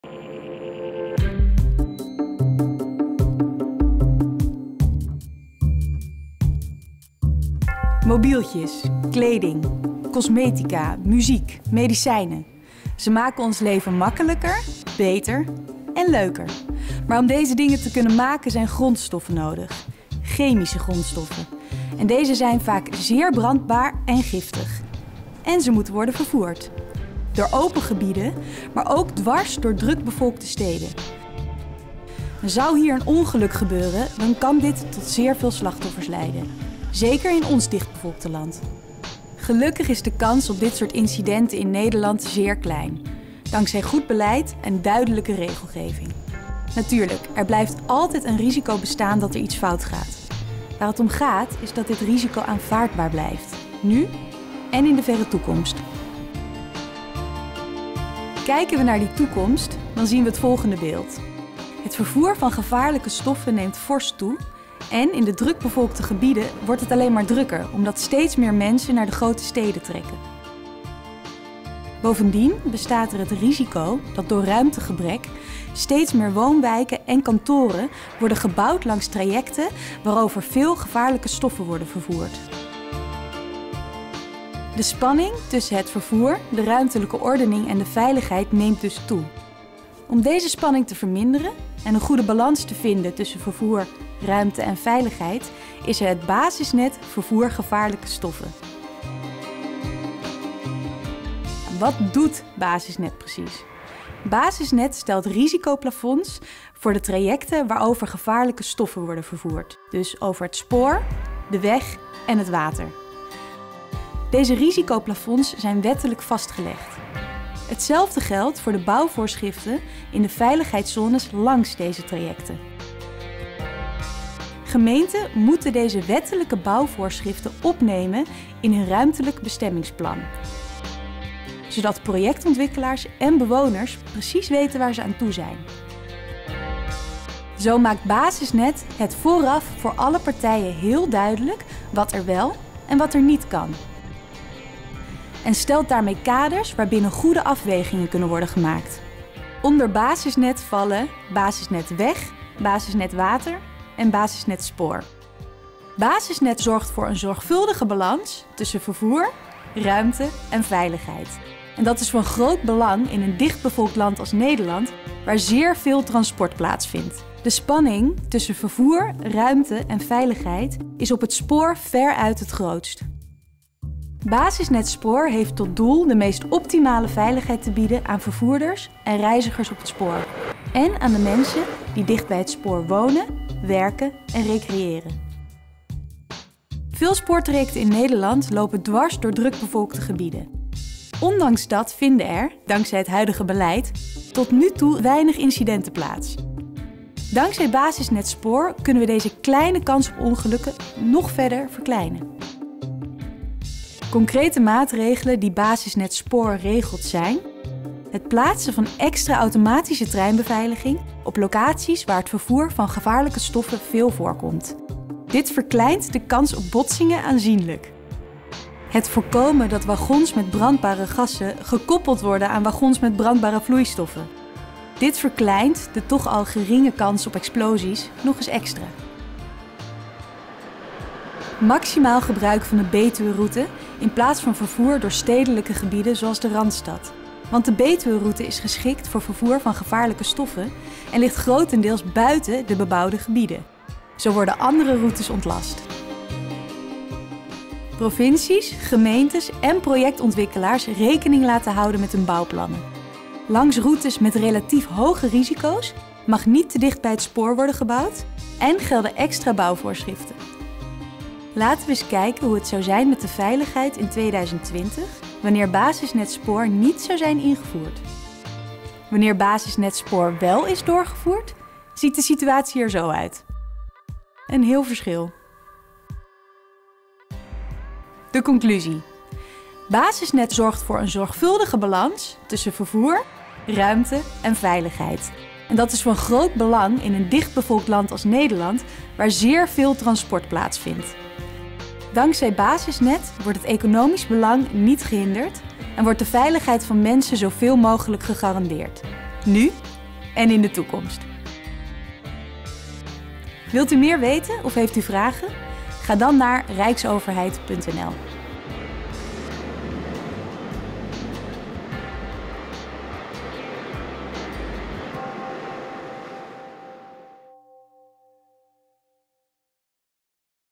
Basisnet - Vervoer gevaarlijke stoffen
Film geproduceerd in opdracht van met Ministerie van I&M, over het vervoer per trein van gevaarlijke stoffen.